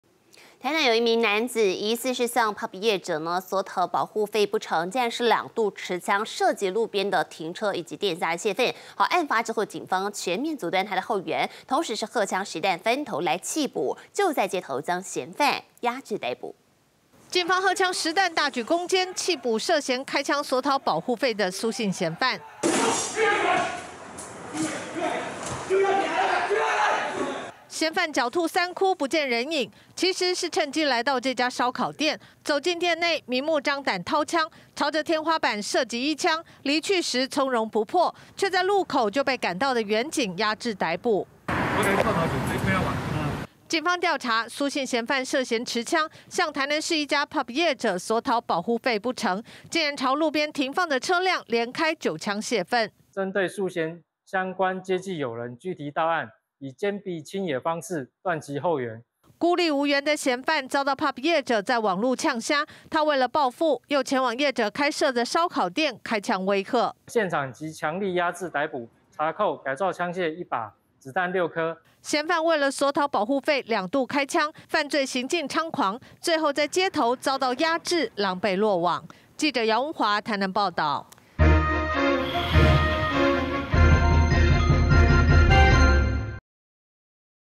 台南槍擊案！ 男闖燒烤店開槍 路邊車也遭狂轟遭殃
台南一名男子疑似向Pub業者索討保護費不成，竟然兩度持槍射擊停放在路邊的車輛，還有店家洩憤，案發後警方全面阻斷他的後援，同時荷槍實彈分頭緝捕，就在街頭將嫌犯壓制逮捕。